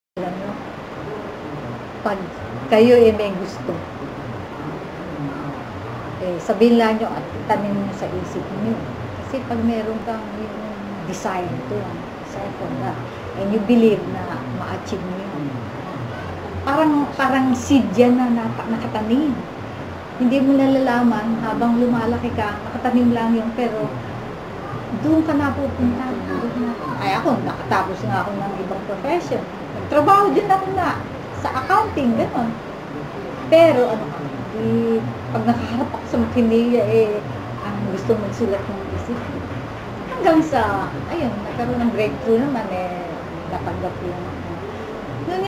Interview-SoFA-Evelyn Sebastian-Tuparin ang pangarap mo
Interview with Palanca Awardee: Evelyn Sebastian